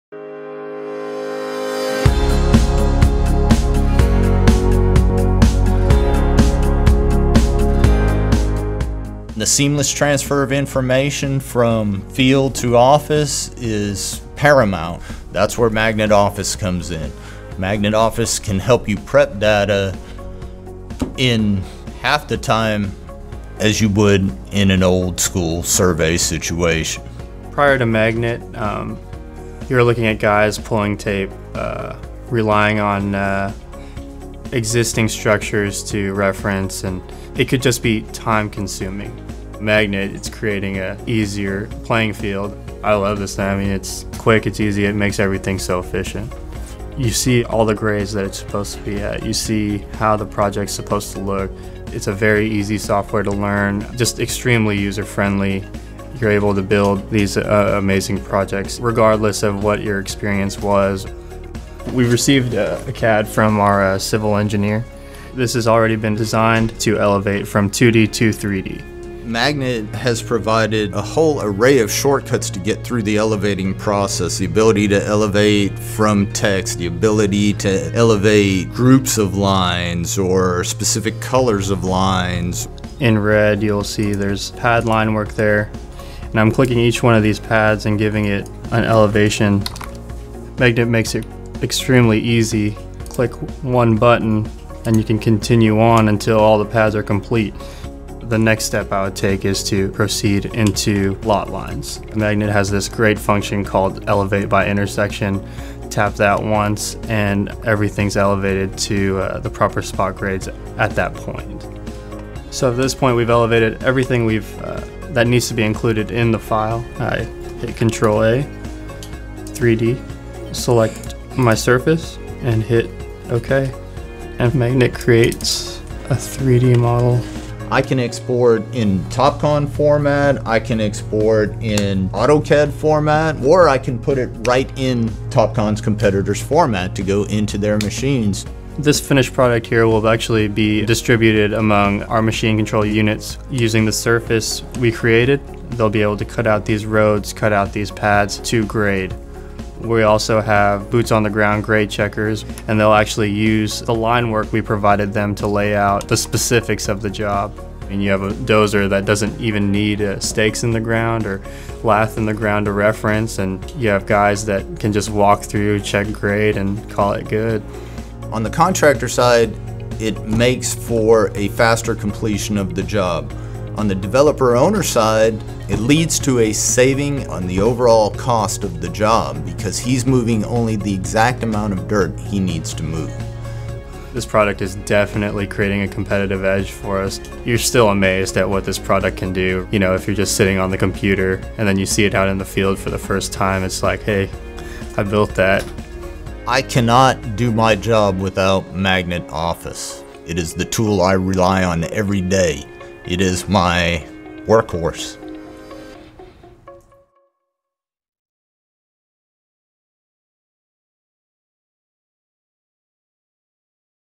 MAGNET Software speeds up workflow of Texas contractors
Witness how MAGNET software seamlessly connects the field to the office, and back again. 3D models can be easily created from a variety of formats, ranging from PDF plans to CAD design files. MAGNET software enables the exporting in a variety of formats to fit the needs of the individual contractor. Regardless of field experience, MAGNET is not only easy to learn, but is an essential tool being utilized daily by Texas-based companies AMI 3D Services and RPM xConstruction.